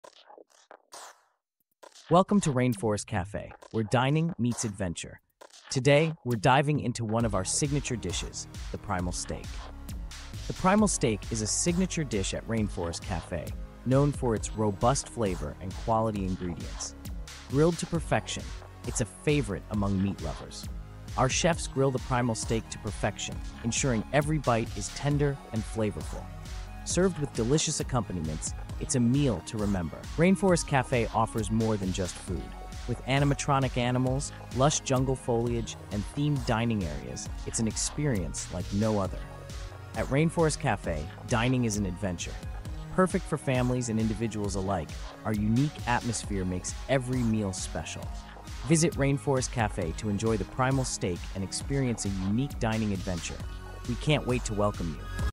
What is primal steak rainforest cafe
Primal Steak Rainforest Cafe is a signature dish offered at Rainforest Cafe, a themed restaurant chain known for its jungle-like ambiance and unique dining experience. The Primal Steak is a hearty and flavorful cut of steak that is grilled to perfection and served with a side of delicious accompaniments. This dish is a popular choice among meat lovers and is often highlighted on the menu for its robust flavor and quality ingredients. Additionally, Rainforest Cafe is renowned for its immersive decor, featuring animatronic animals, jungle foliage, and themed dining areas that transport guests to a tropical rainforest setting. The restaurant provides a fun and entertaining dining experience for families and individuals looking to enjoy a meal in a unique and adventurous atmosphere.